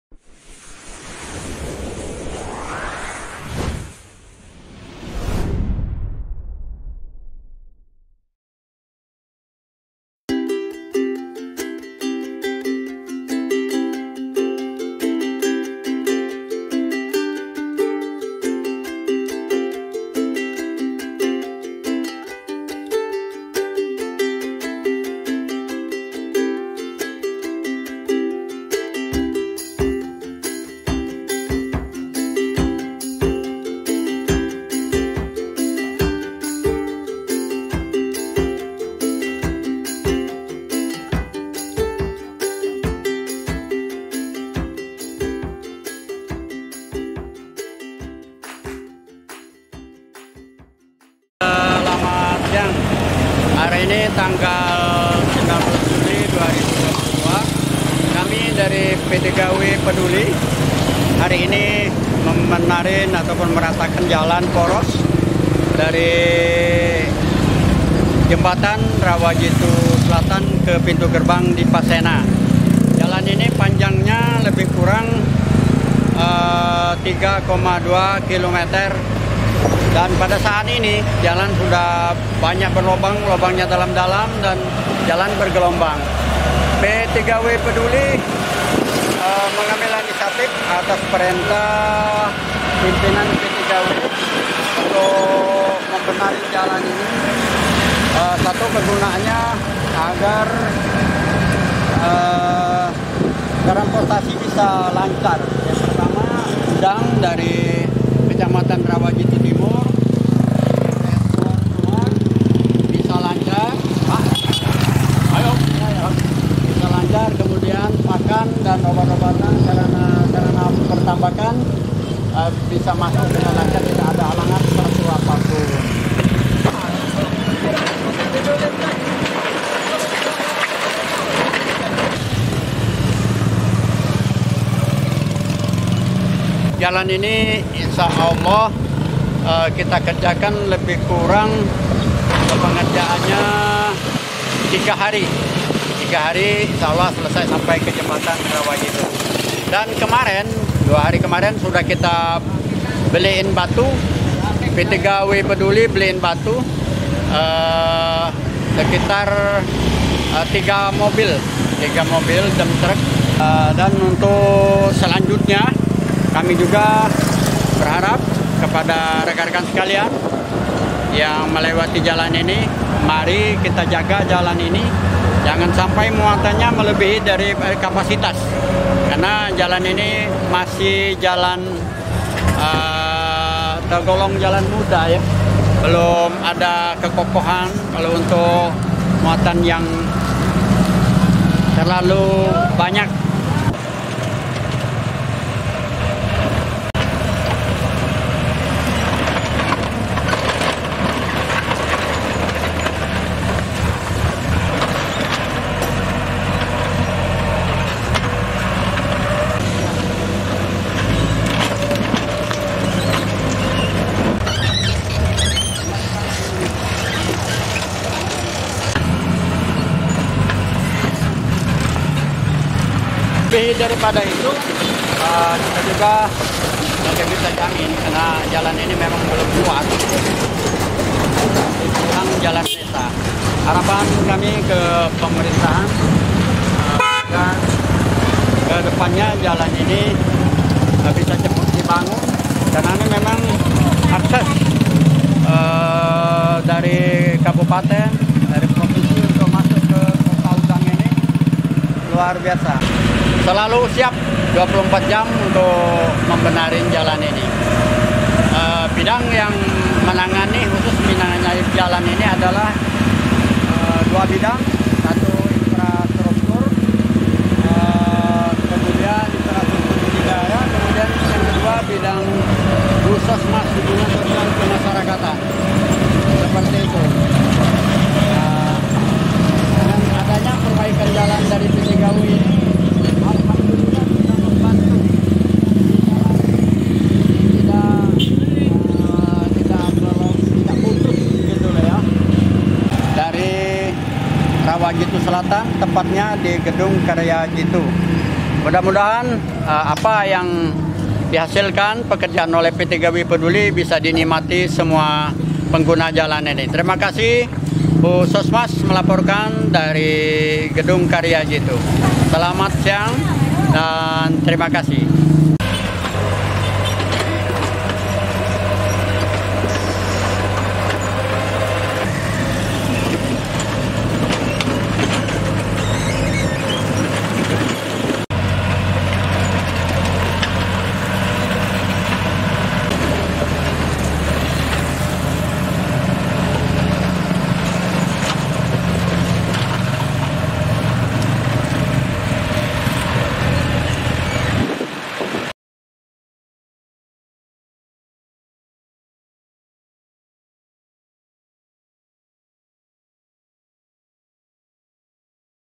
PERBAIKI JALAN RAWAJITU,GREDER DITURUNKAN P3UW LAMPUNG
Perbaikan jalan poros Rawajitu (Pintu gerbang tambak Dipasena - Jembatan pasar Rawajitu) kecamatan Rawajitu Selatan Kabupaten Tulang Bawang, Lampung, 19 - 21 Juli 2022.

Perbaikan dilakukan oleh P3UW Lampung dengan menurunkan Alat berat Greder.